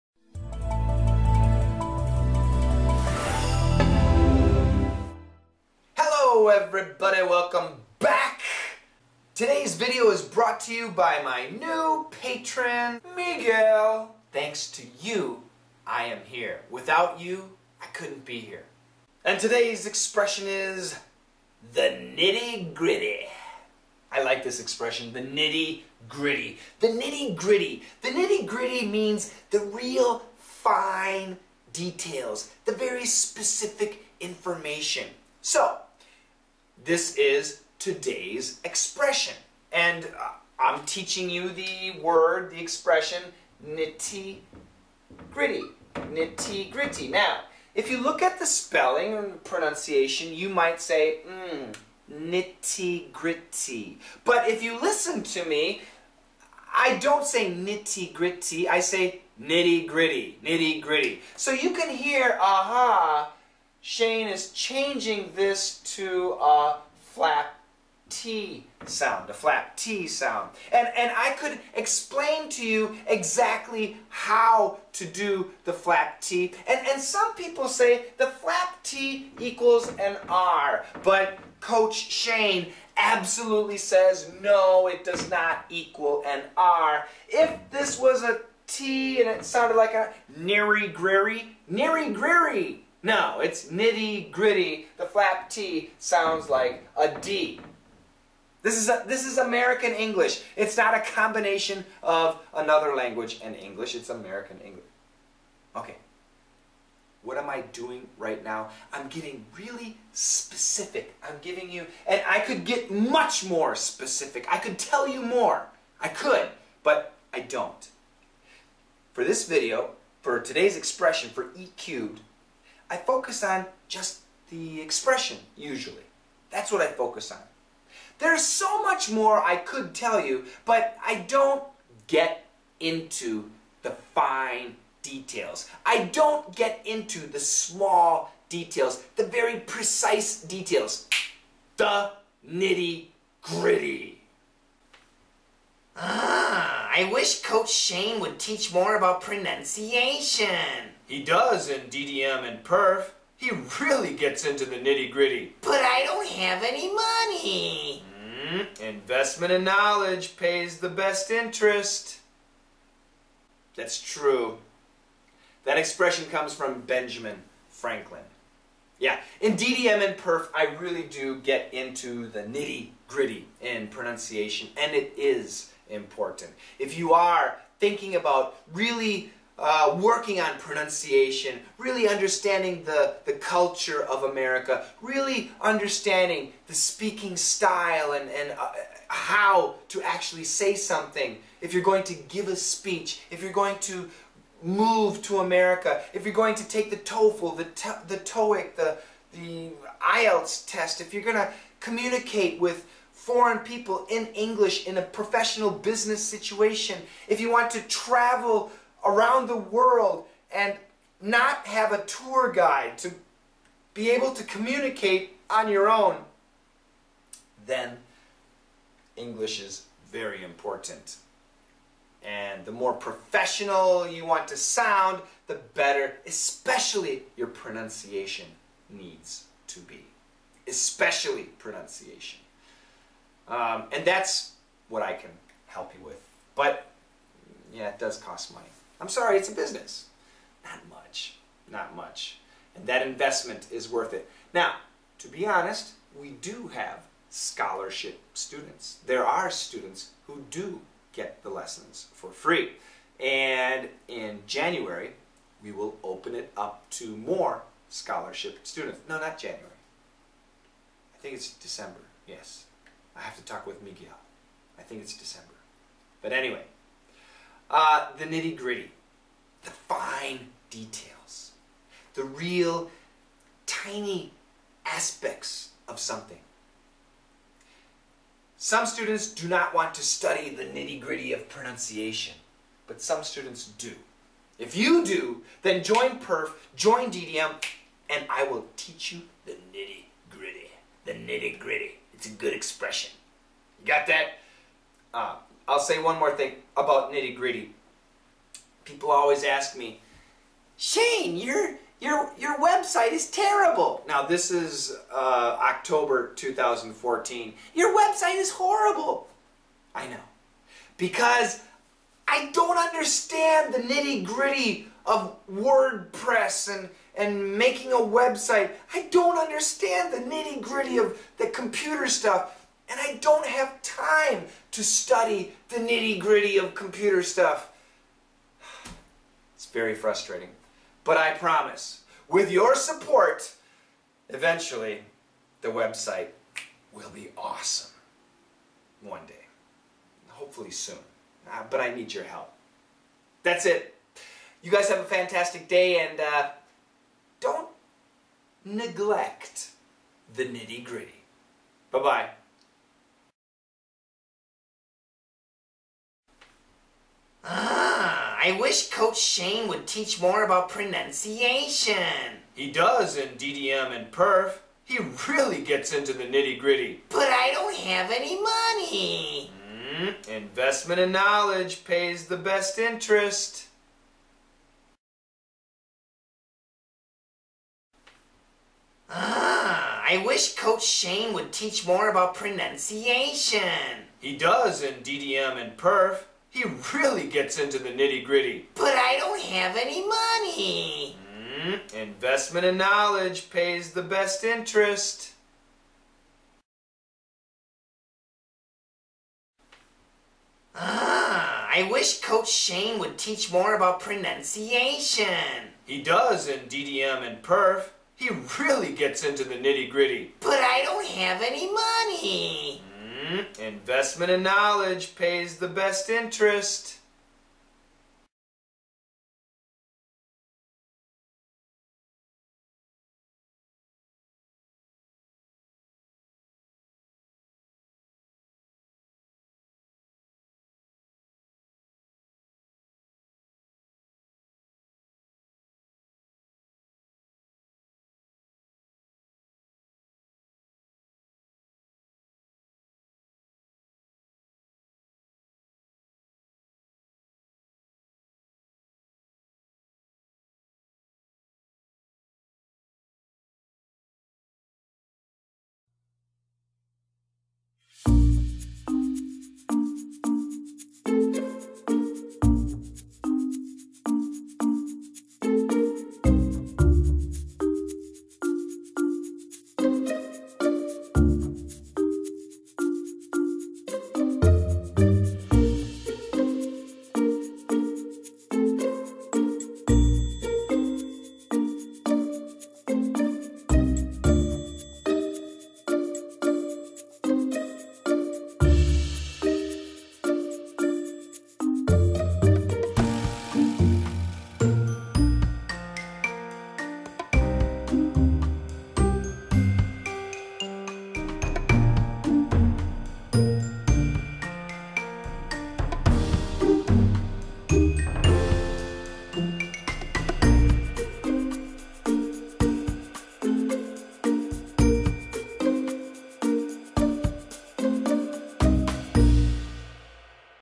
Learn English: Daily Easy English Expression 0601: the nitty-gritty
Today's E-cubed: the nitty-gritty
I wish Coach Shane would teach more about pronunciation.
He does in DDM and PIRF. He really gets into the nitty-gritty.
But I don’t have any money.
Investment in knowledge pays the best interest.

This is a video for ESL (English as a Second Language) learners. This video explains a common expression in American English. It also explains typical American pronunciation.